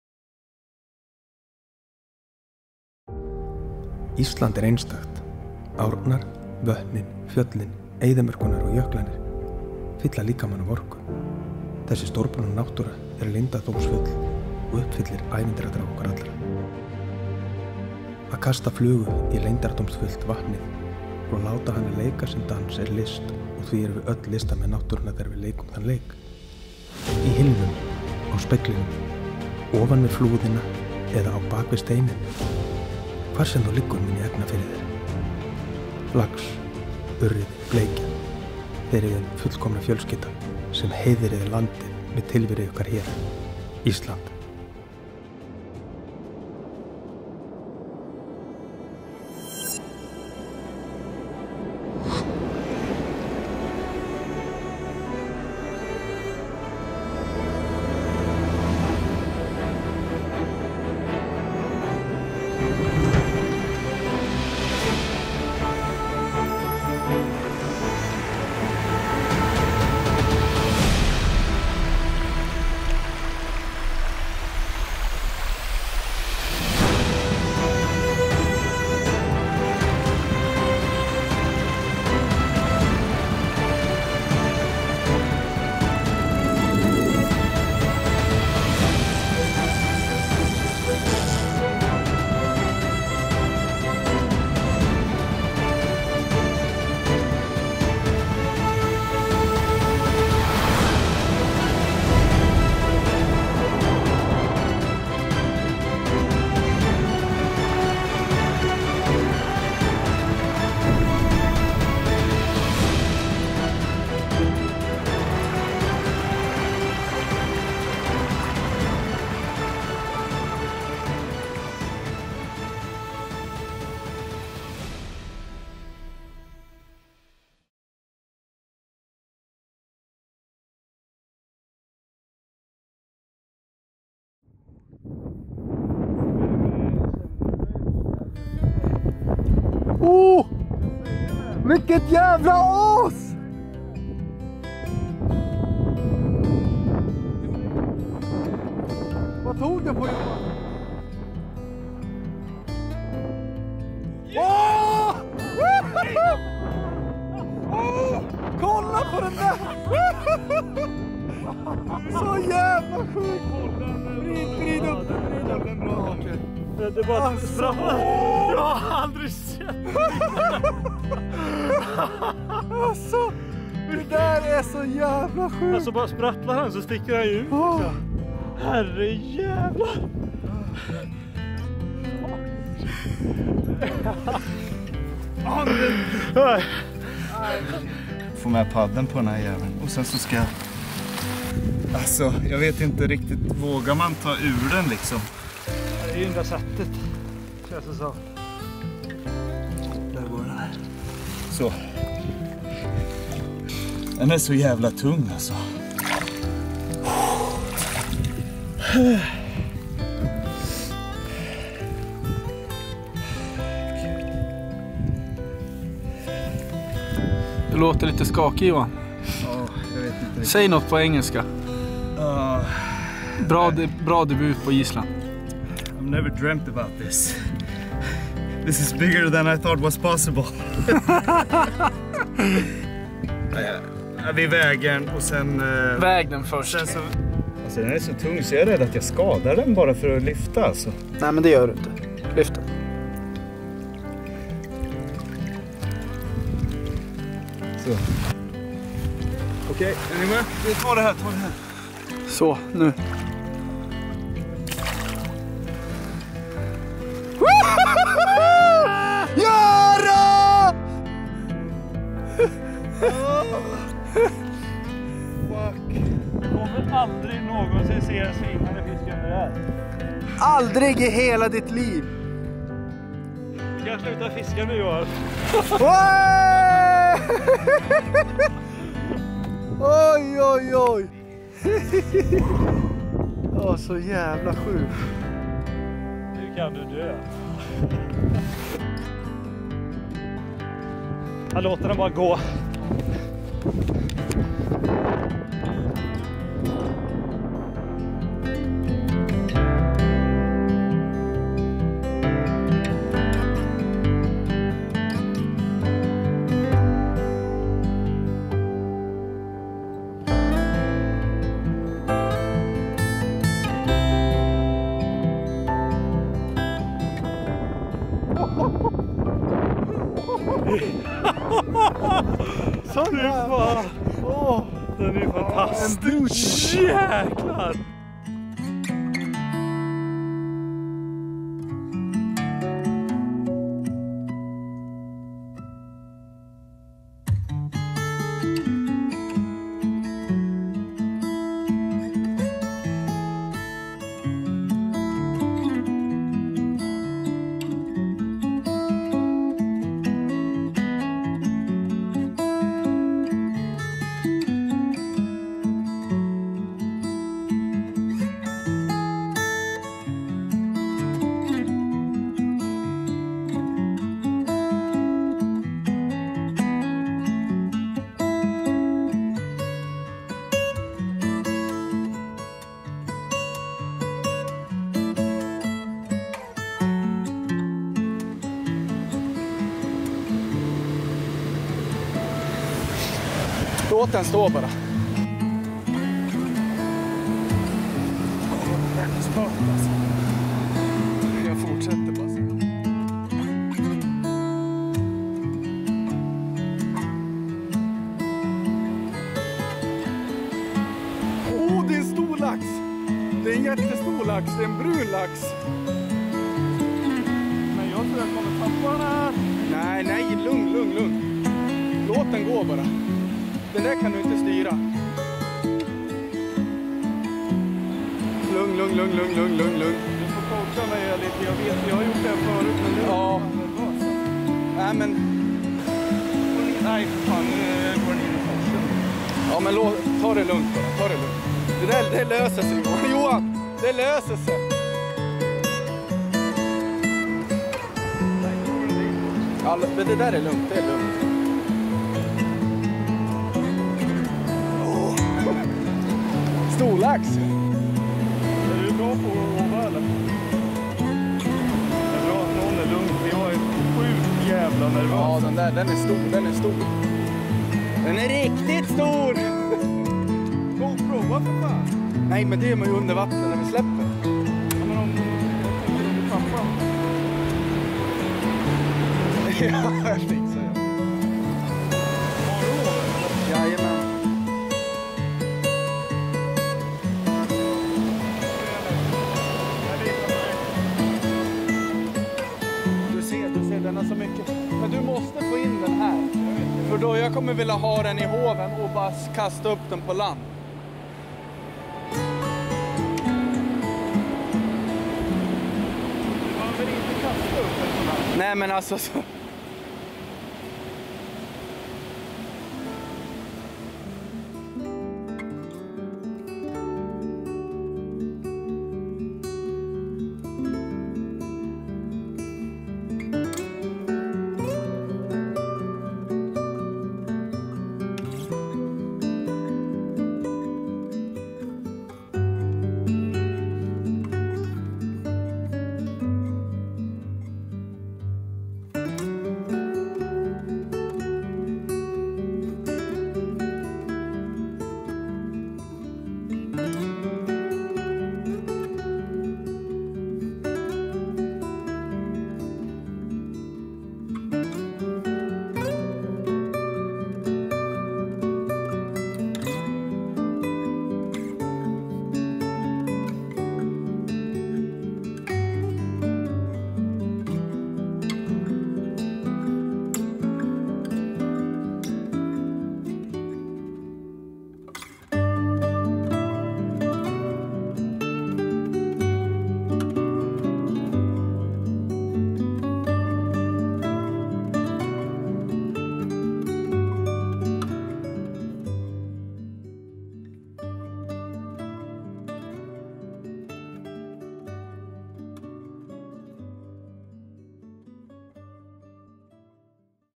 FLY FISHING in Iceland : Ice Age Brown trout in Villingavatn, Arctic char in Kaldakvisl
Spent one week in Iceland with my friends Johan and Tommie looking for our dream fish. Here is the result!

Gear used:

Sony A6500
Sony E PZ 18-105/4 G OSS

Edited in Adobe Premier Pro